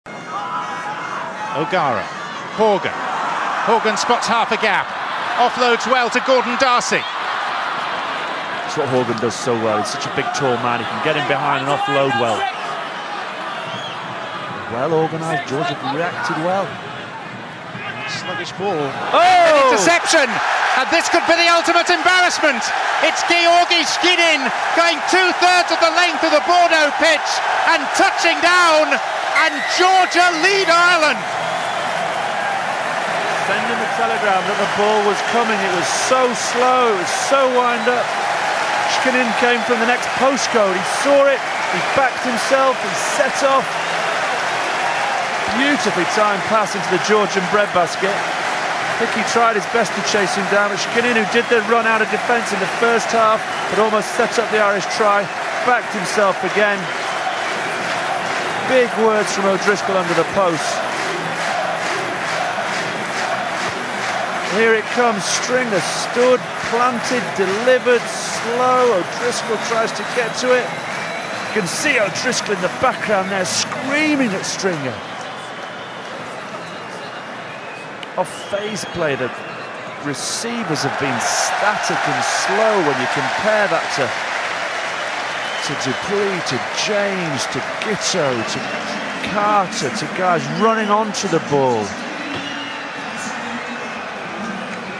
"And Georgia lead Ireland"
Georgian winger Giorgi Shkinin comes "from the next postcode" to intercept a pass from Peter Stringer and score under the posts to give his side a lead against Ireland in the 2007 Rugby World Cup.